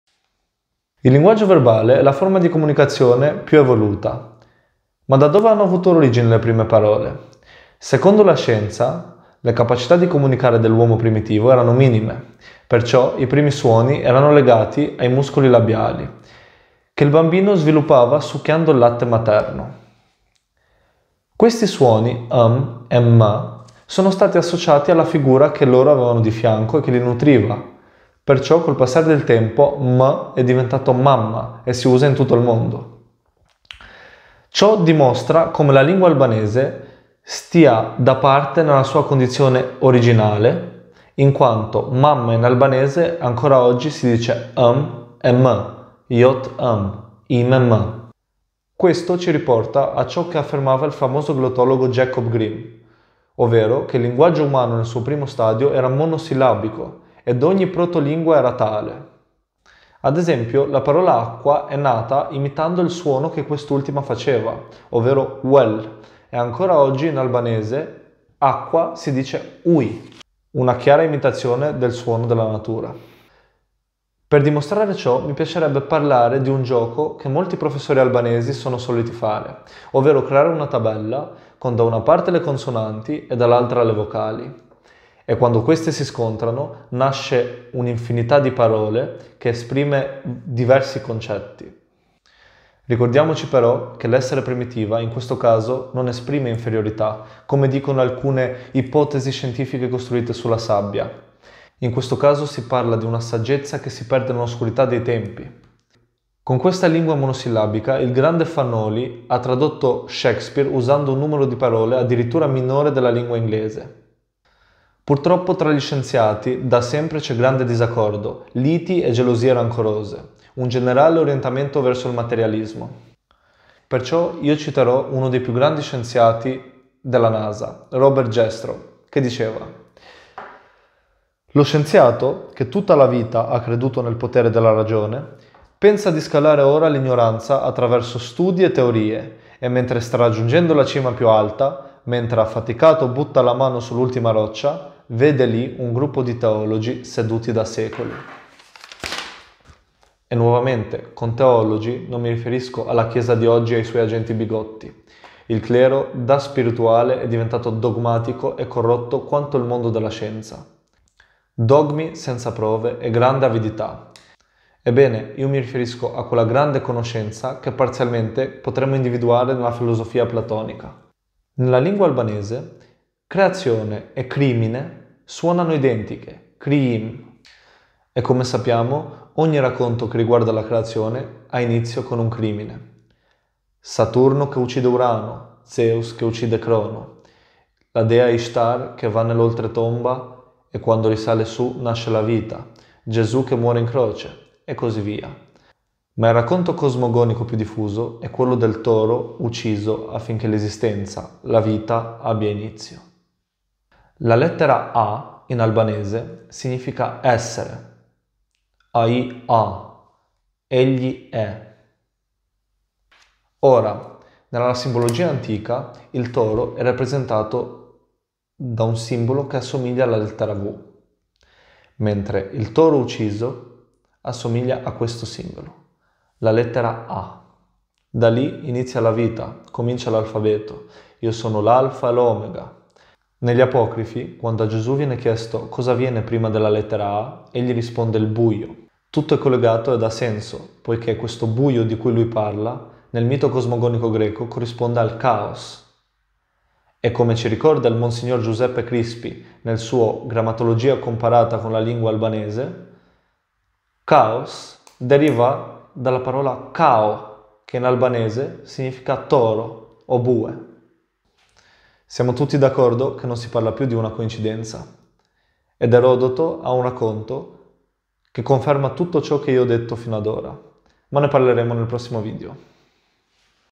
Tra scienza e teologia: La lingua albanese
Nëse dëshironi të ndihmini për kërkimet dhe përhapjen e gjuhës dhe kulturës tonë:

-Evidenze scientifiche sull'antichità della lingua albanese confermate dai miti cosmogonici in tutto il mondo.

*saggio di grammatologia comparata sulla lingua albanese - di Demetrio Camarda
*Memorie sulla lingua albanese di Giuseppe Crispi
Un ringraziamento al prof. S. Giaquinto e al grande prof. G. Leka.